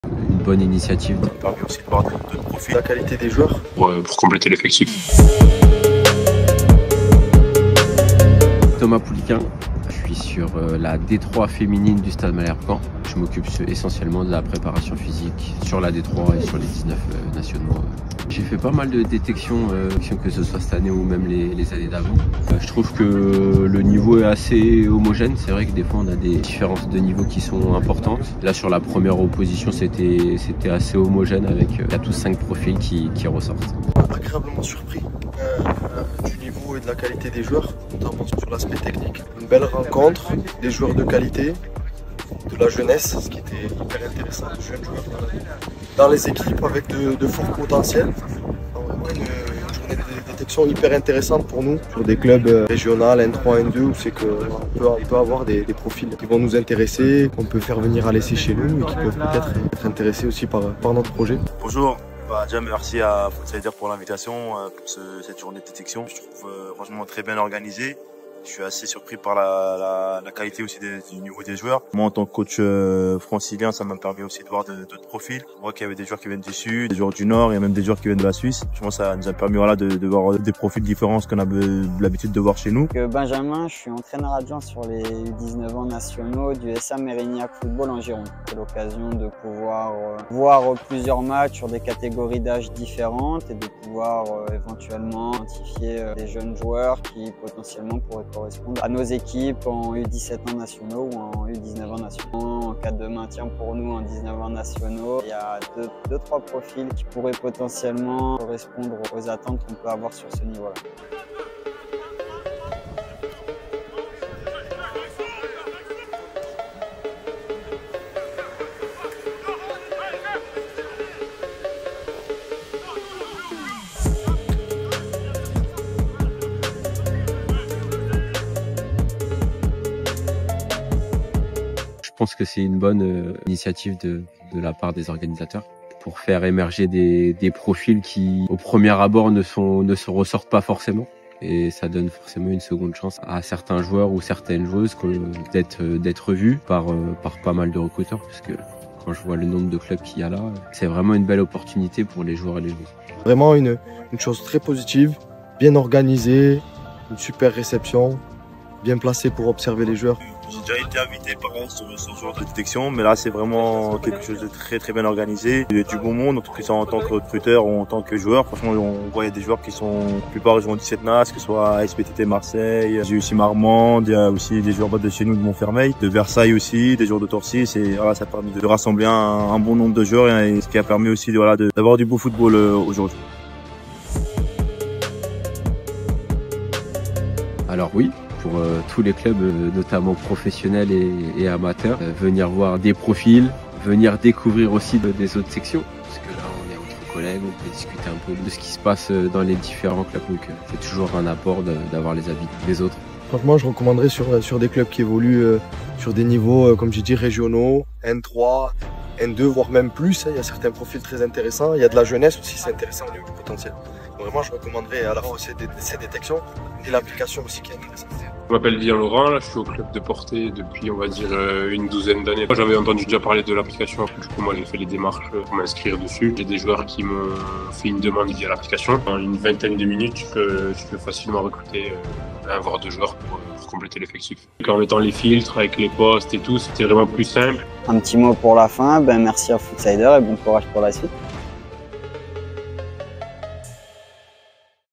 Retours de COACHS après une DÉTECTION
Lors de notre Scouting Day en avril 2024, de nombreux coachs de club de football étaient présents pour détecter de nouvelles pétites. Dans ce récapitulatif, retrouvez les avis de ces coachs sur les performances des joueurs présents, l'organisation du Scouting Day ainsi que sur l'utilisation de Footsider.